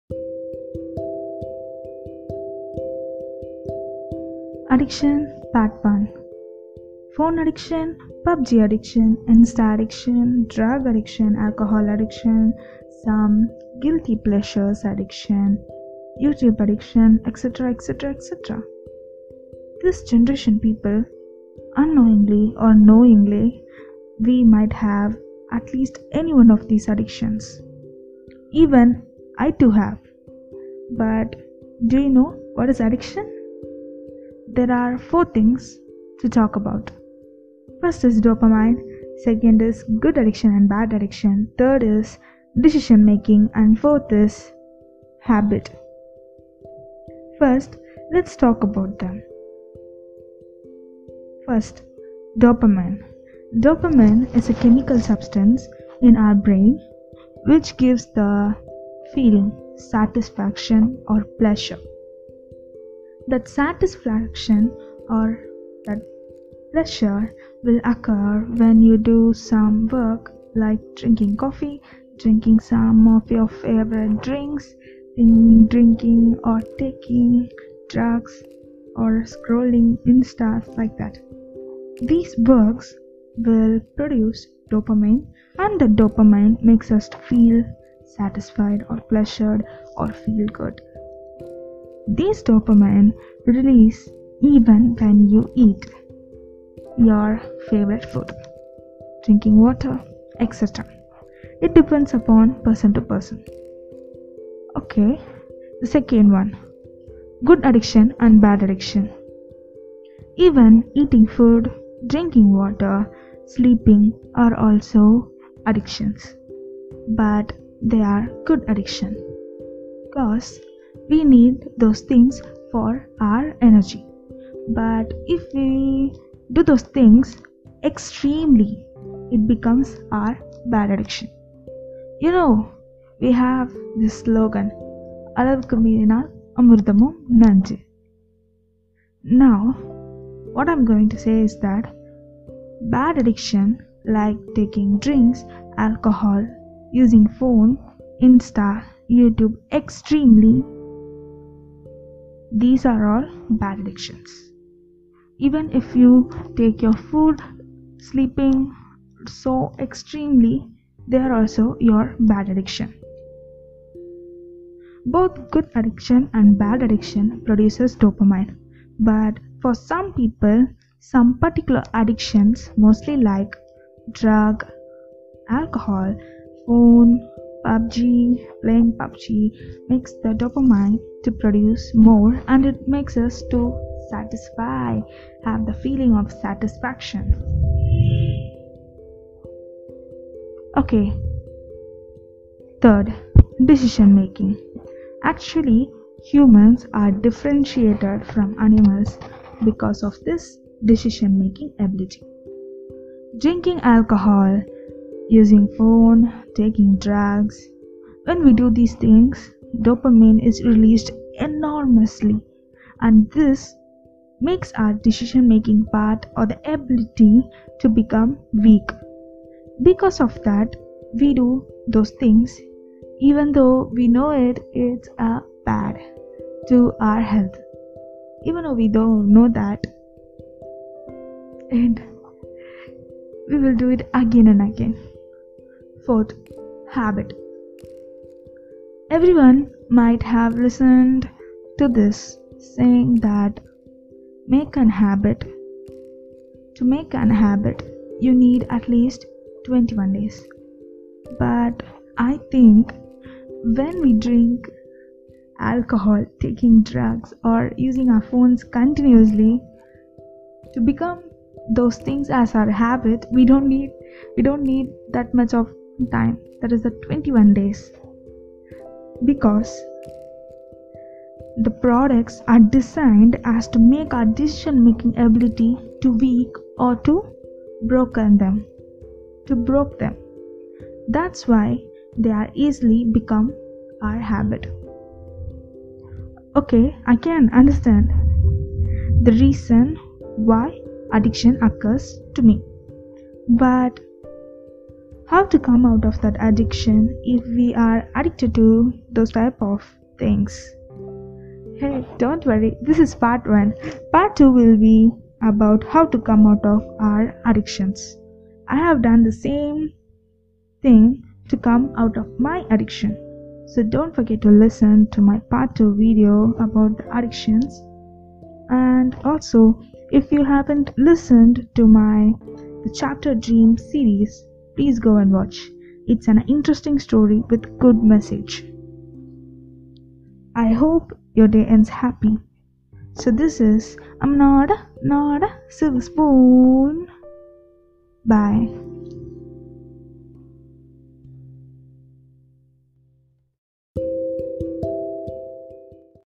A D D I C T I O N ( P A R T 1 )
In this video u are going to see about addiction . How this addiction occurs? reason ? Like many things are said in this podcast / video. I hope u enjoy this video.

This Podcast / Youtube channel is always for HEALING OTHERS.

I HOPE YOUR DAY ENDS HAPPY
BYE